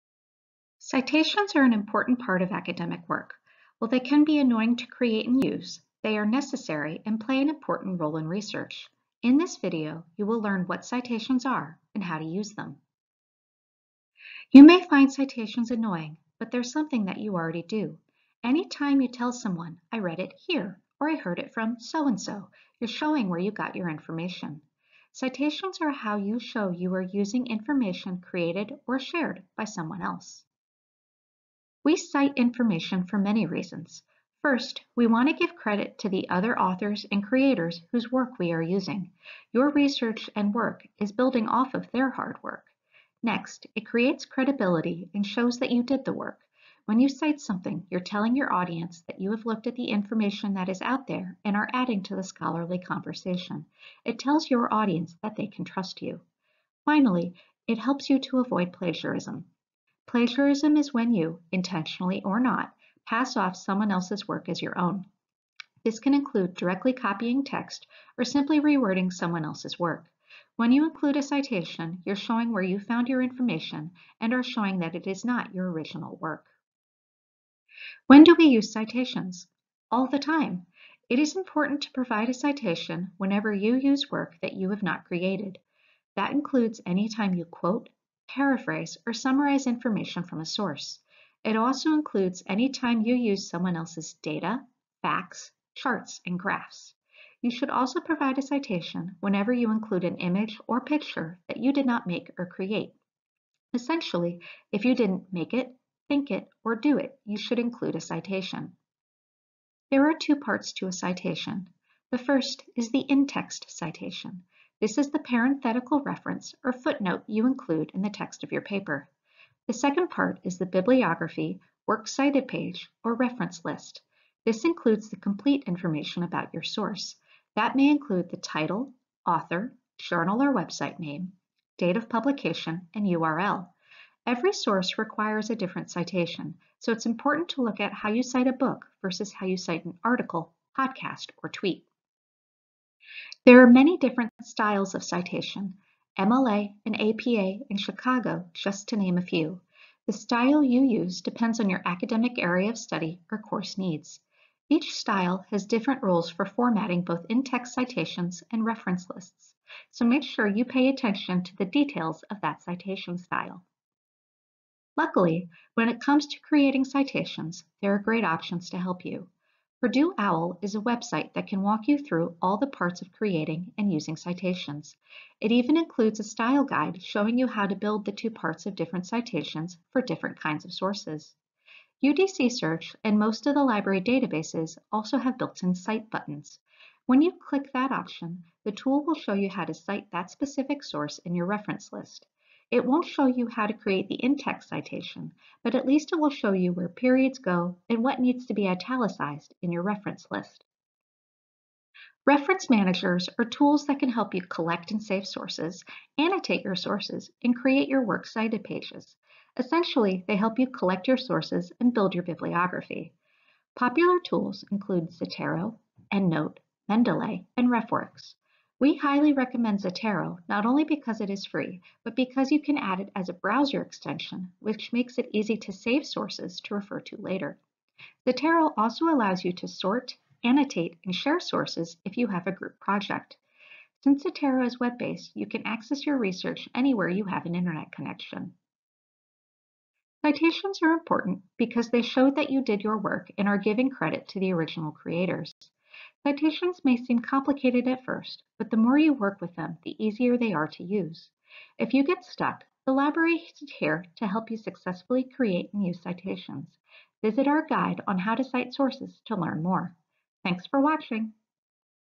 Citations
Citations are an important part of academic work. While they can be annoying to create and use, they are necessary and play an important role in research. In this video, you will learn what citations are and how to use them.